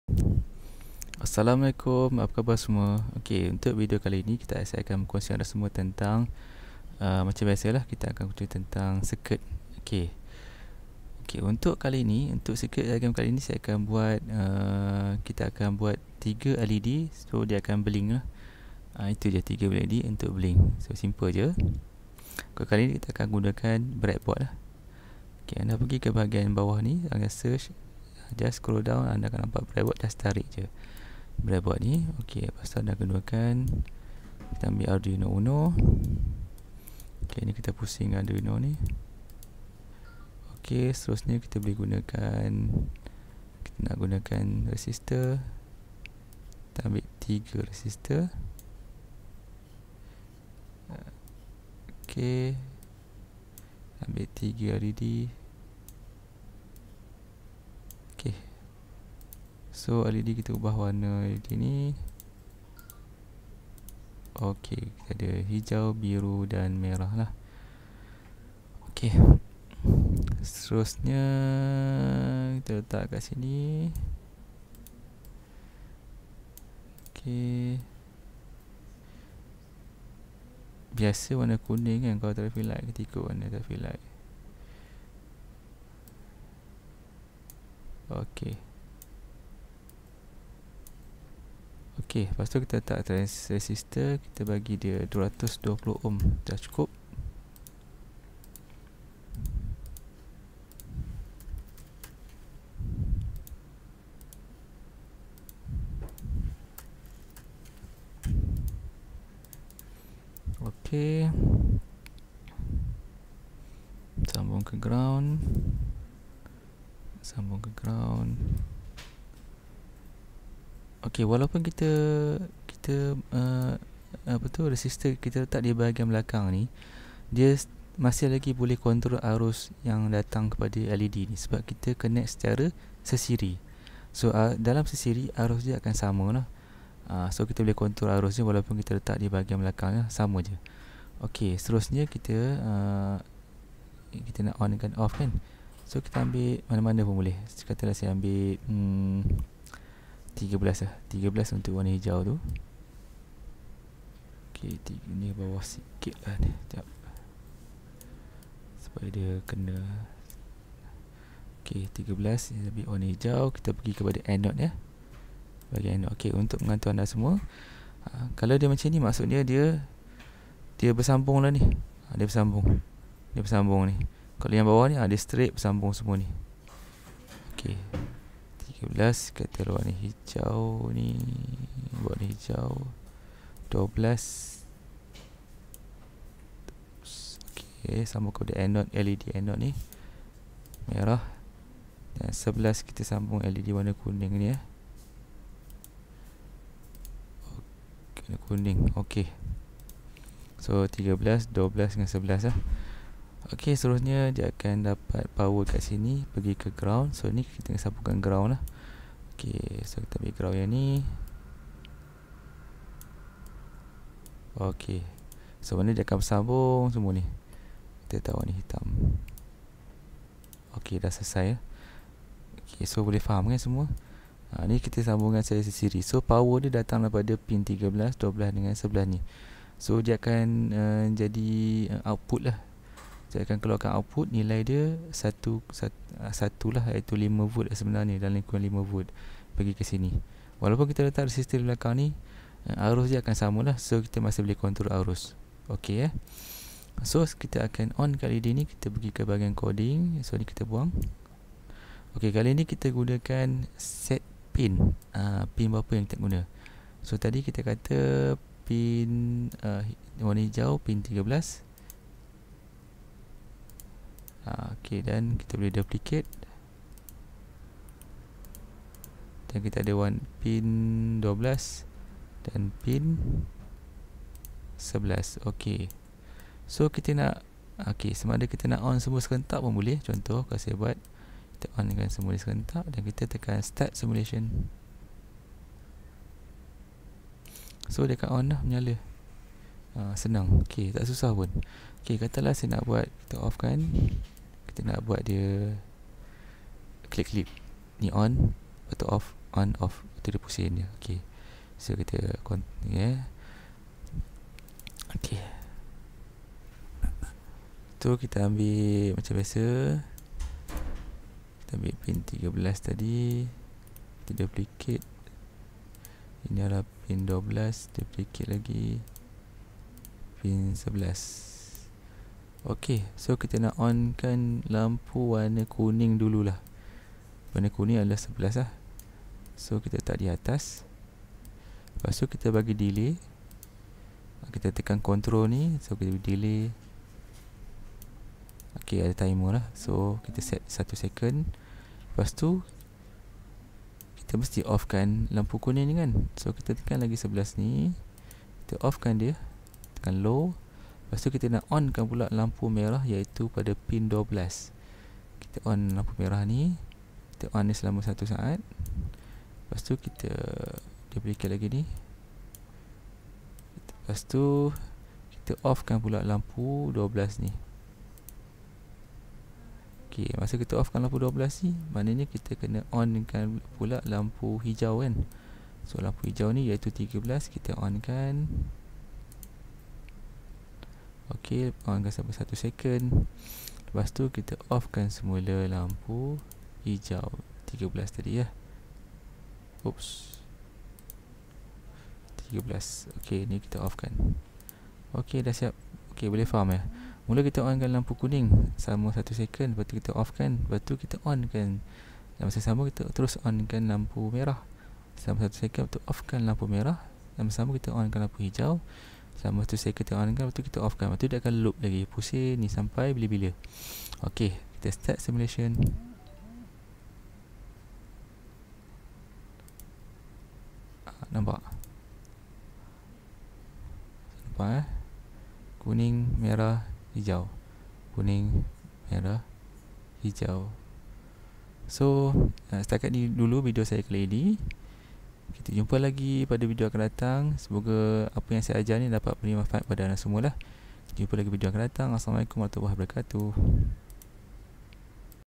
BLINK 3 LED oleh EN SYAZWAN
Video ini disediakan oleh En Syazwan dari Jabatan Matematik, Sains dan Komputer, Politeknik Ungku Omar.
Video ini menerangkan cara untuk membuat 3 lampu LED berkelip menggunakan Tinkercad.